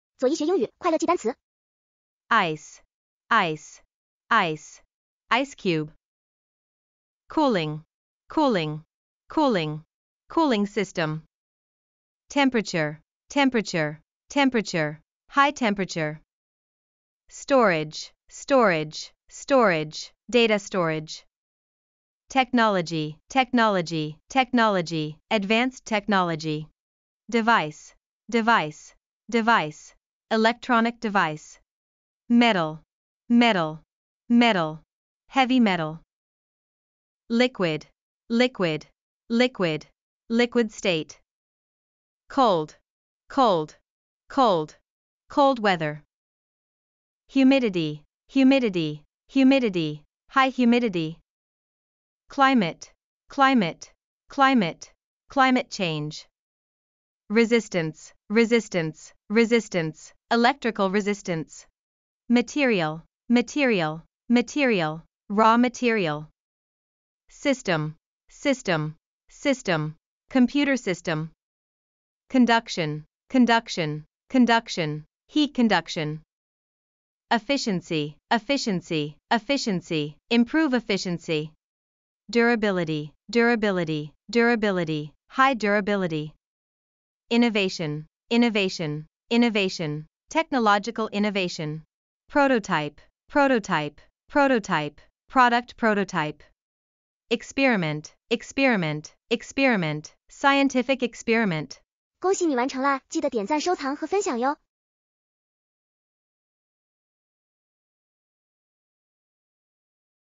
用冰做硬盘应对变暖♥️热词赏
本视频围绕“用冰做硬盘应对变暖”主题，介绍 ice（冰）、cooling（冷却）、temperature（温度）、storage（存储）等核心词汇，讲解利用低温材料和先进技术提高硬盘耐热性与效率的创新实验和原型设计，帮助学习者理解气候变化背景下存储设备的技术挑战与解决方案。